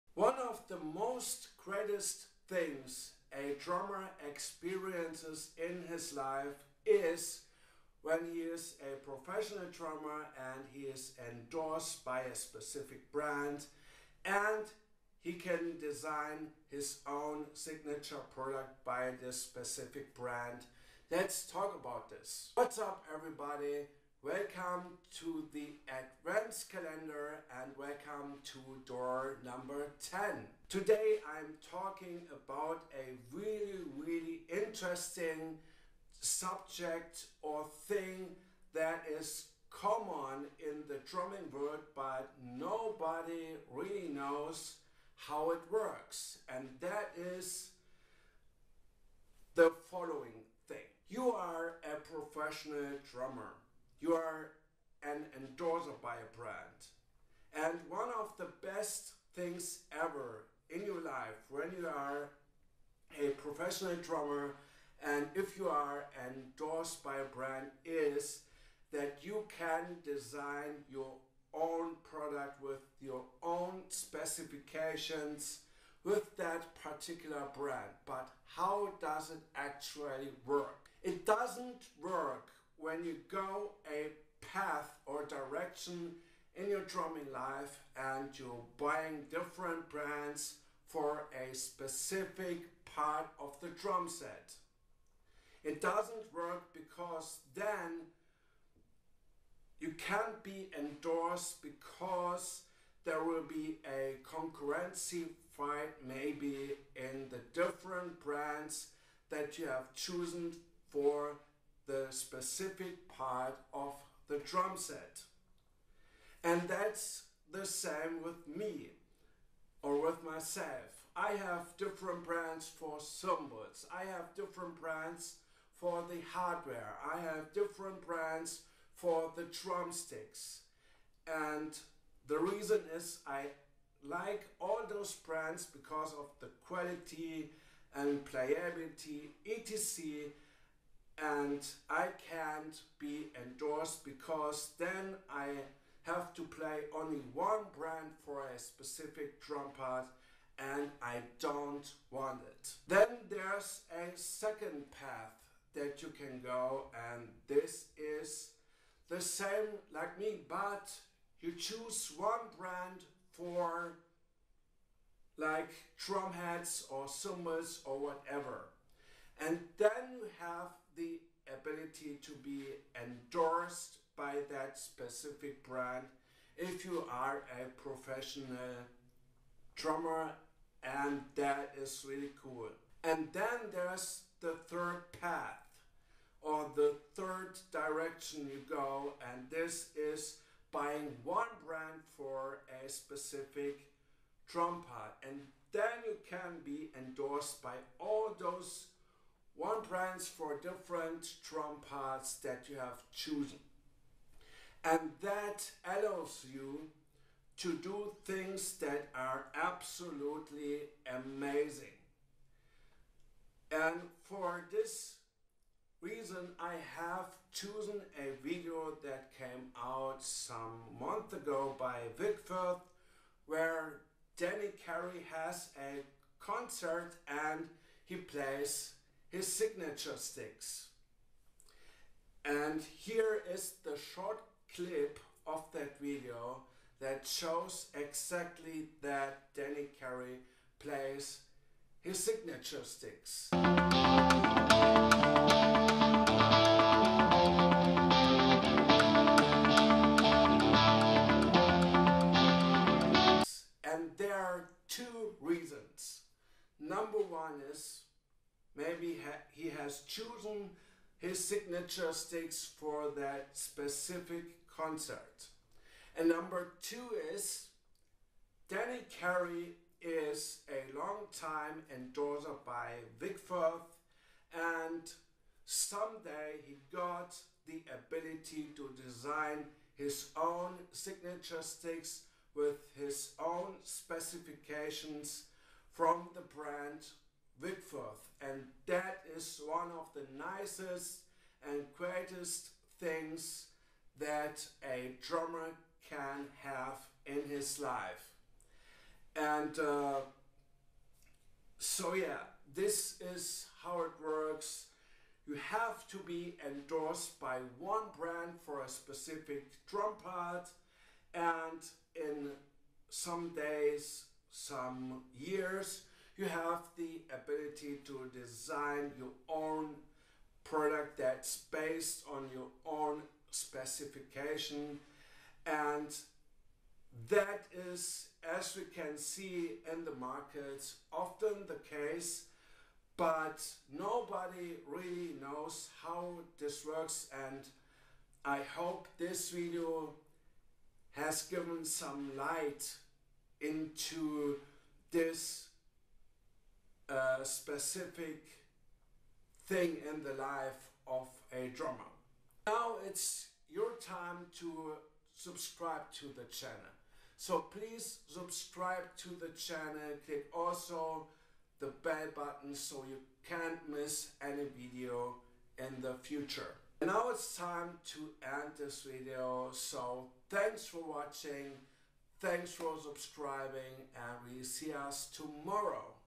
Advents Calender Door 10-Signature Products. What Are They?
Signature Products made by brands and musicians are one of the best things ever. But how does it work? how can a musician or a drummer have a signature product? this video  lights that topic or issue up

full vic firth video of the performance of dany carey

Sorry for  my bad english in this video. its not always simple to say what you want in a foreign language